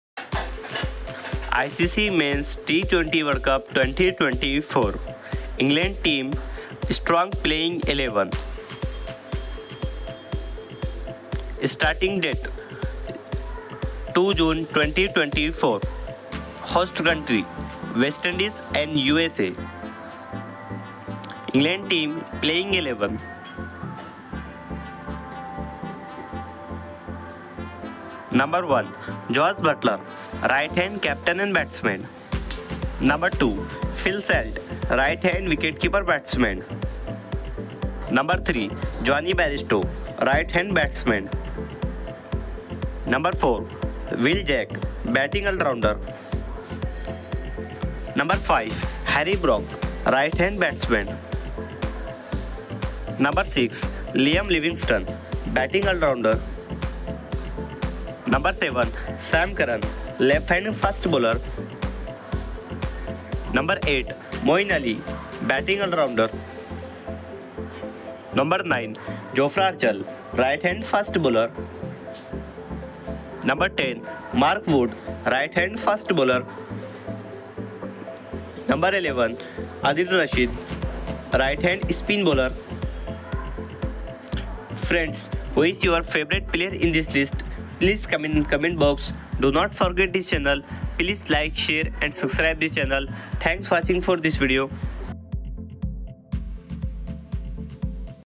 T20 World Cup 2024 | England Team Playing 11 2024
Jos Buttler (C), Phil Salt, Jonny Bairstow, Will Jacks, Harry Brook, Liam Livingstone, Sam Curran, Moeen Ali, Jofra Archer, Mark Wood, Adil Rashid,

◆LIKE ◆SHARE ◆SUBSCRIBETHANKS FOR WATCHING